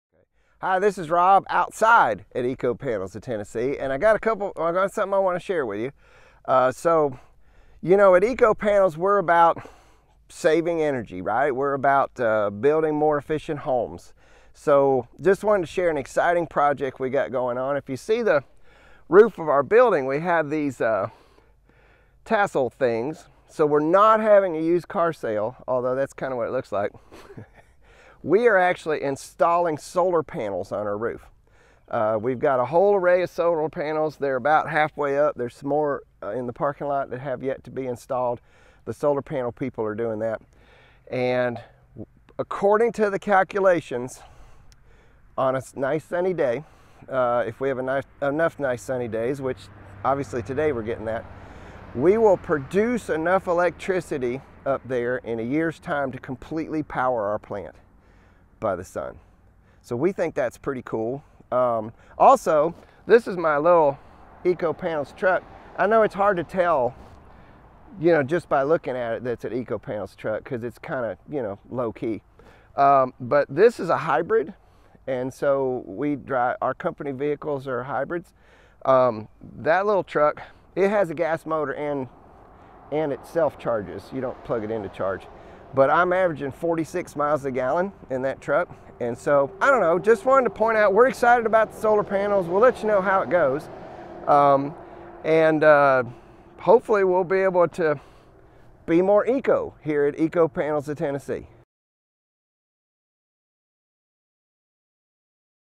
Solar Panel Project with Eco-Panels of Tennessee
Get an exclusive insider's look into the future of sustainable energy as Rob, our esteemed Vice President at Eco-Panels of Tennessee, unveils our latest solar panel project. 🏭 In this video, Rob shares his vision for integrating solar power into our manufacturing process, highlighting the environmental benefits and technological advancements driving this initiative forward. Join us as we embark on this journey towards a cleaner, greener tomorrow. Watch now and discover how Eco-Panels is leading the charge in renewable energy innovation! ☀️